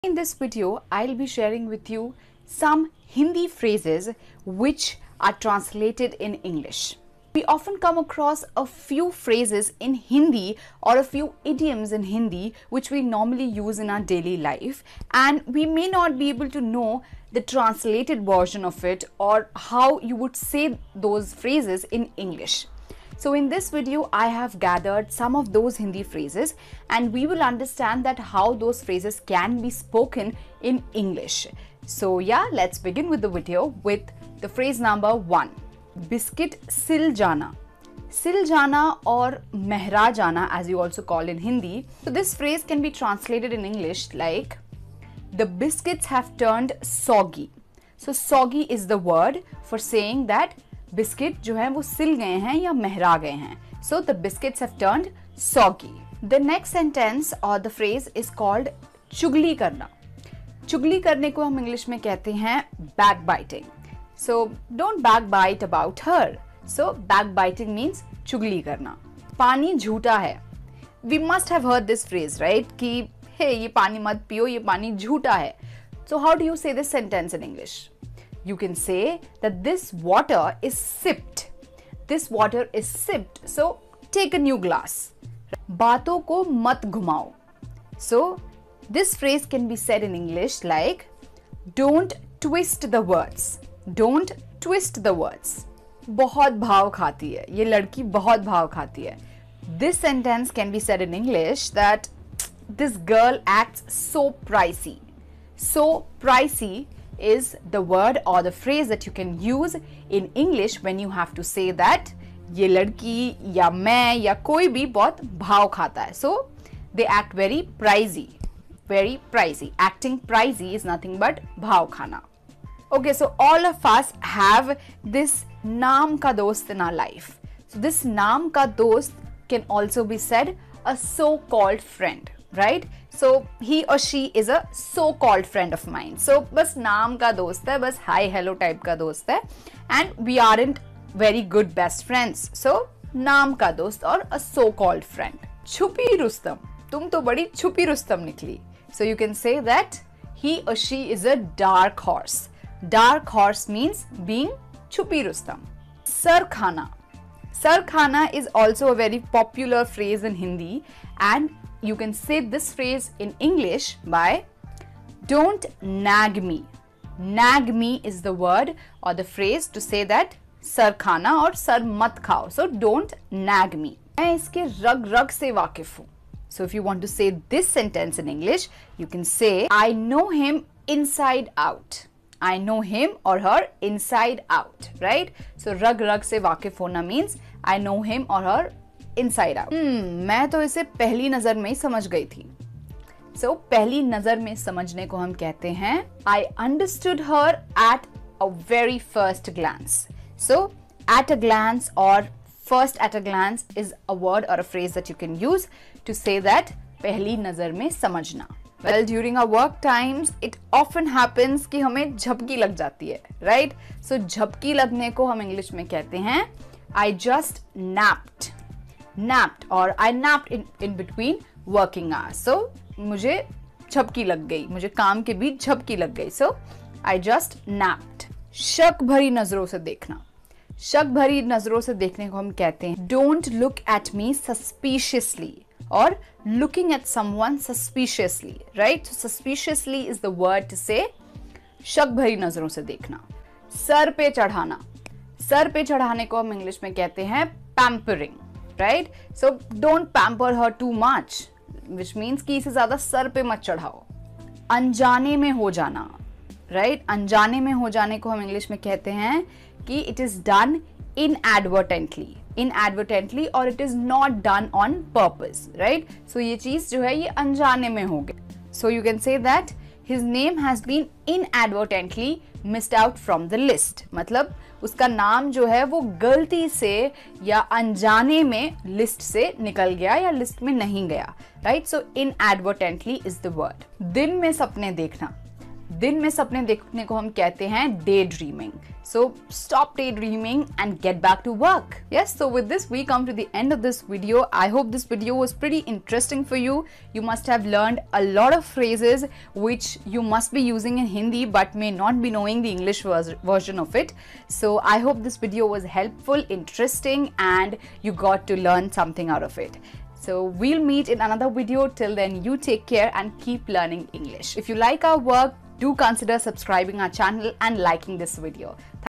Common Hindi phrases in English | Learn English
Common Hindi phrases in English | Learn English with Puja Sharma
1. Biscuit sil jaana
2. Chugli karna
3. Paani jhoota hai
4. Baato ko mat ghumao
5. Bhav khaana
6. Naam ka dost
7. Chupi rustom
8. Sir khana
9. Rag rag se wakif hona
10. Pehli nazar me samajna
11. Jhapki lagna
12. Shak bhari nazro se dekhna
13. Sir pe chadhana
14. Anjane me hona
15. Din me sapne dekhna